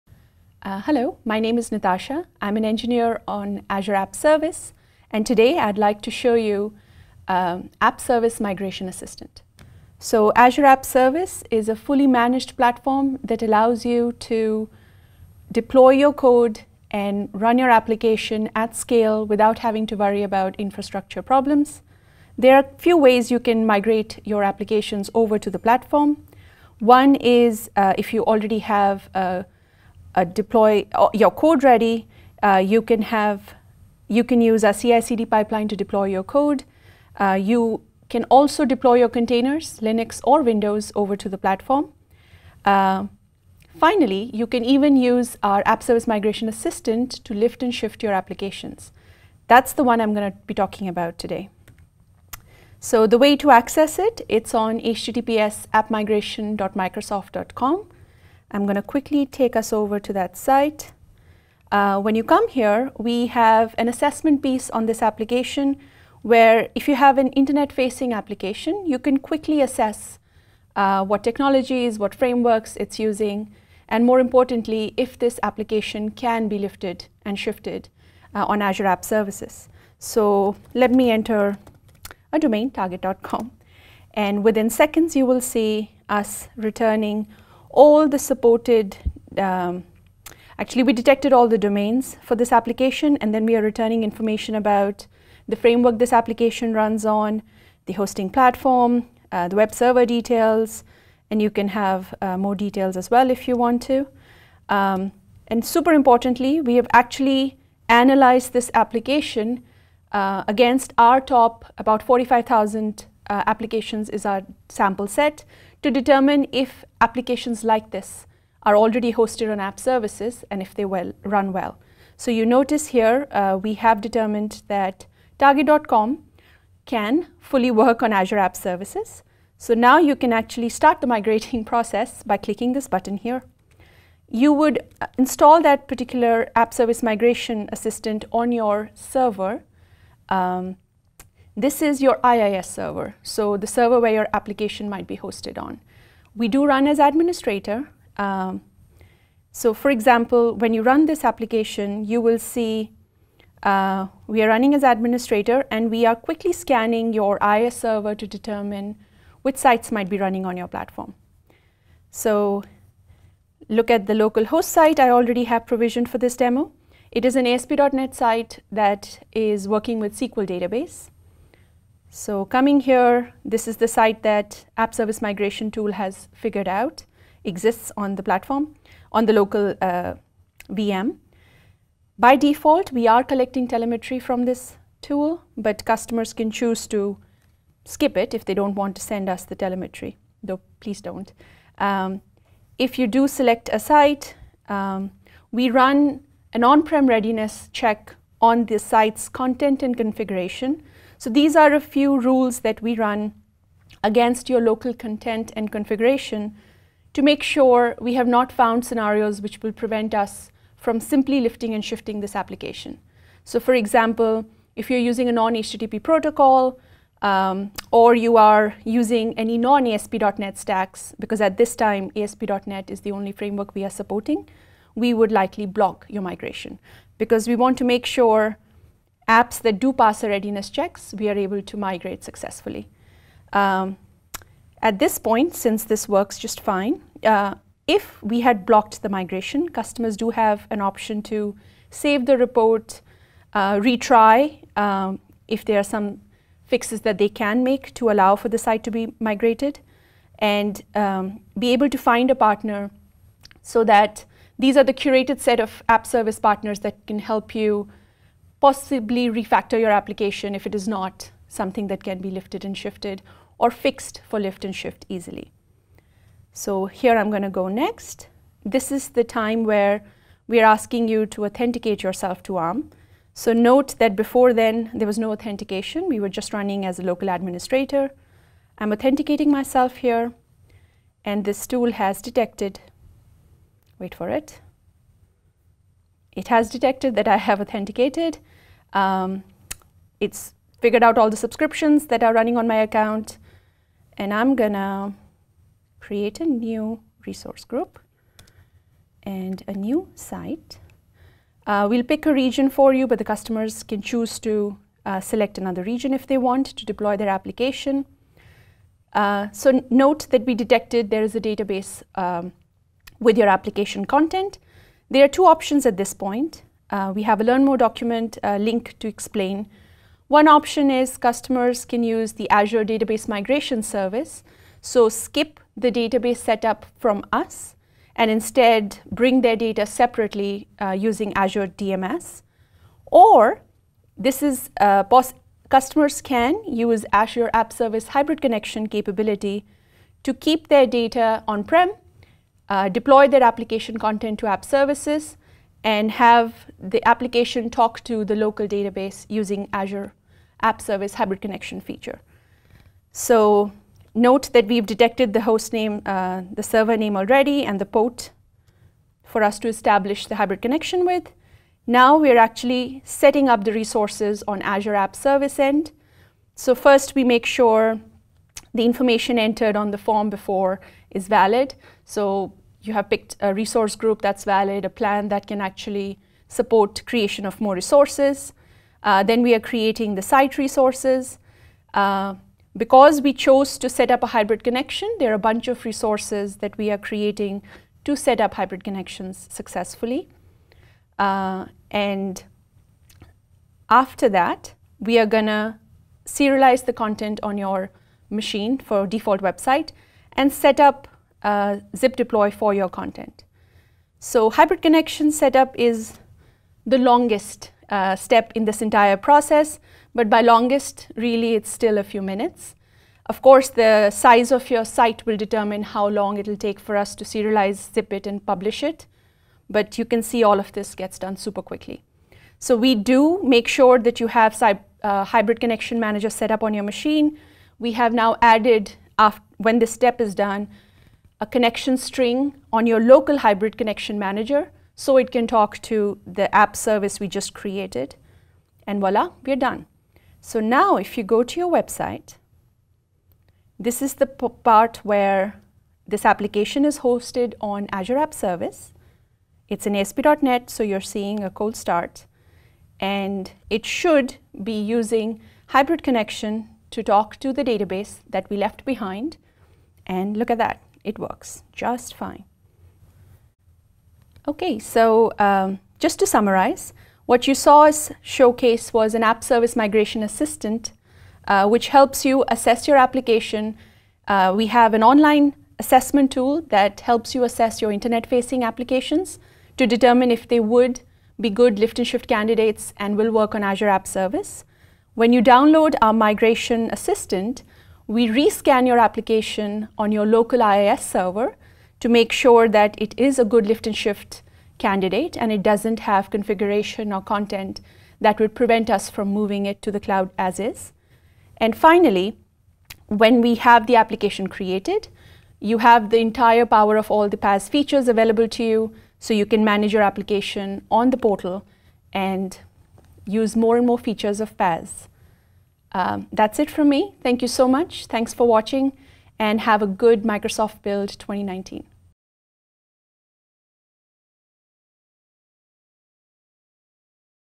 Migrate Your .NET Web Our New Step-By-Step Assistant - PRE10
Move your ASP.Net App or Site to Azure app service with the help of the new Azure App Service Migration Assistant, a step-by-step guide for your assessment, migration and app optimization:
• Assess: Quickly and easily determine if a public endpoint is a good candidate for Azure App Service.
• Migrate: Download the Migration Assistant to perform a detailed assessment of your ASP.Net site and then use the tool to quickly and easily migrated your content and config to Azure App Service.
• Optimize: Explore a dedicated migration experience in the Azure Portal that analyzes your apps and provides detailed configuration guidance.